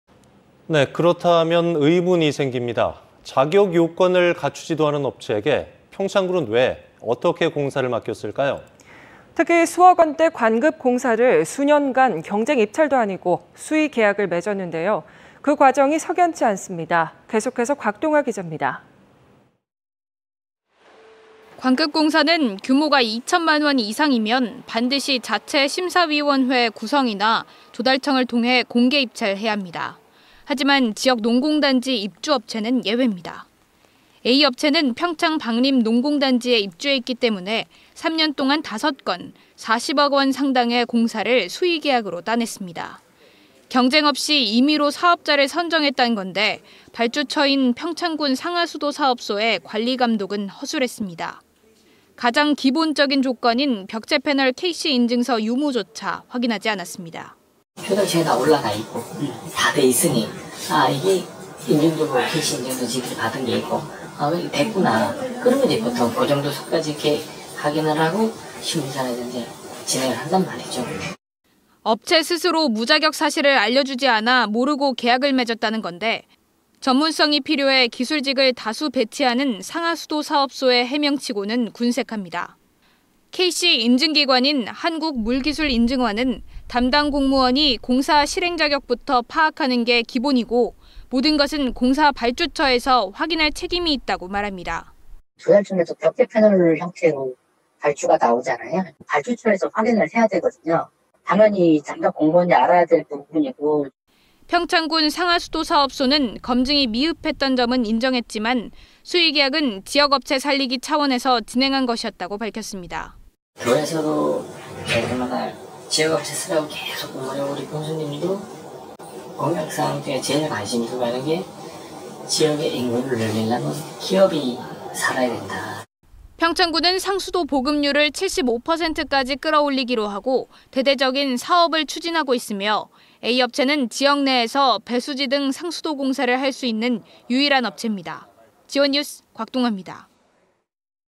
[G1뉴스] 자격검증 없이 수년간 수의계약 '왜?'
그렇다면 의문이 생깁니다. 자격 요건을 갖추지도 않은 업체에게 평창군은 왜, 어떻게, 공사를 맡겼을까요.

 특히 수억원대 관급공사를 수년간 경쟁 입찰도 아니고 수의계약을 맺었는데요, 그 과정이 석연치 않습니다.